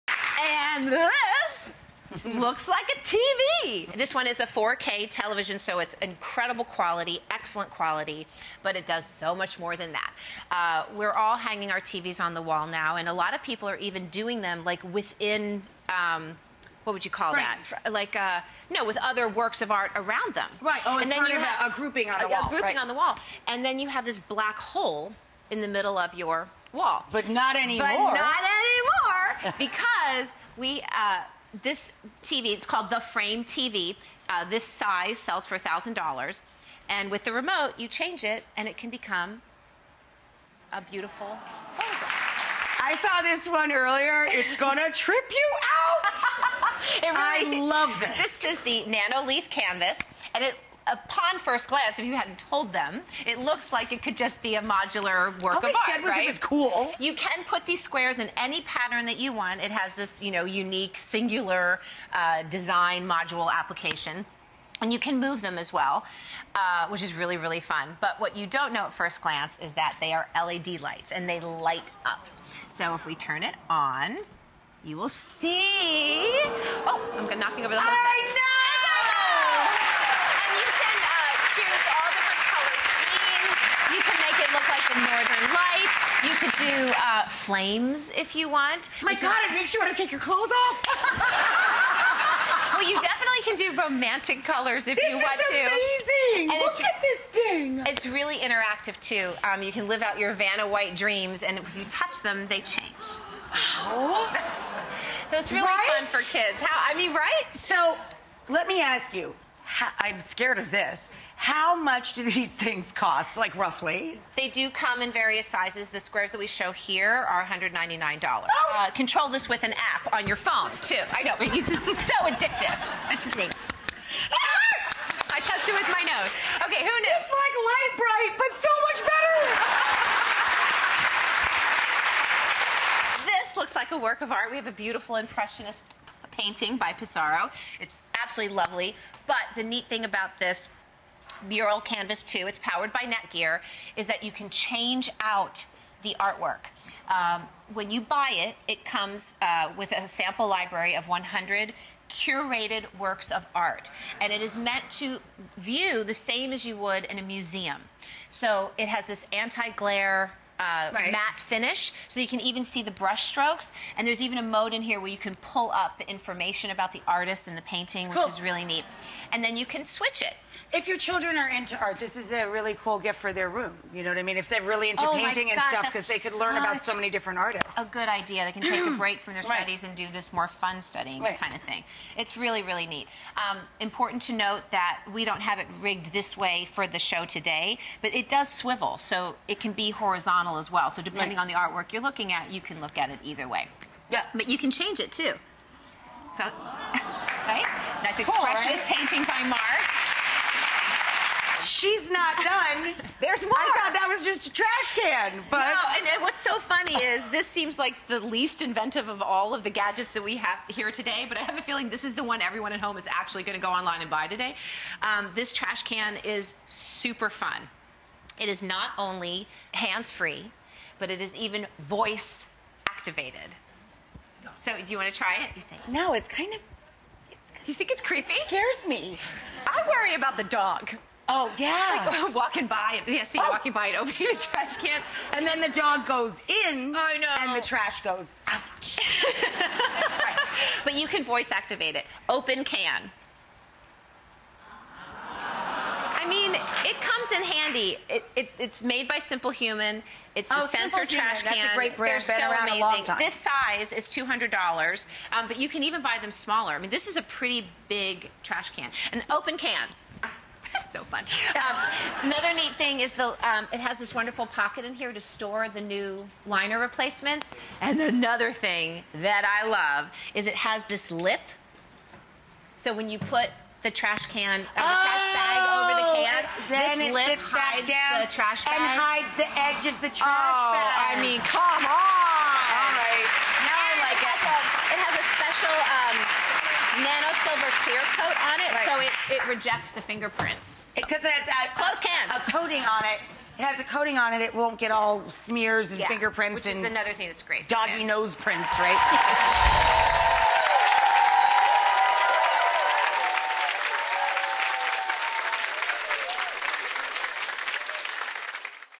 Frame TV Demo + 3 Other Cool Smart Home Technology Products | Trading Spaces Host Paige Davis
Frame TV + 3 other gadgets that use smart technology to upgrade your home décor, including high-tech lighting and a voice-controlled, touch-free trash can.